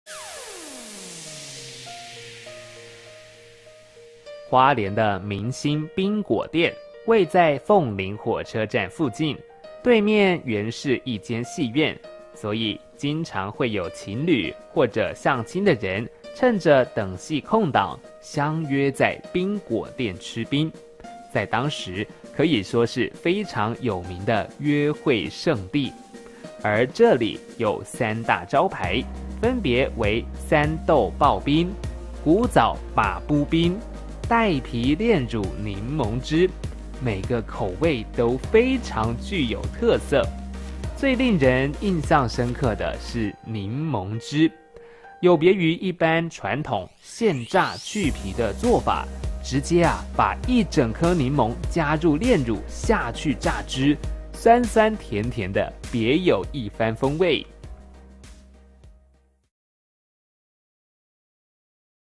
[花蓮縣] 明新冰果店 (美食)
花蓮的明新冰果店，位在鳳林火車站附近，對面原是ㄧ間戲院，所以經常會有情侶或者相親的人趁著等戲空檔，相約在冰果店吃冰，在當時可以說是非常有名的約會聖地，而這裡有三大招牌，分別為三豆刨冰、古早叭噗冰、帶皮煉乳檸檬汁，每個口味都非常具有特色，最令人印象深刻的是檸檬汁，有別於ㄧ般傳統現榨去皮的做法，直接把ㄧ整顆檸檬加入煉乳下去榨汁，酸酸甜甜的別有一番風味！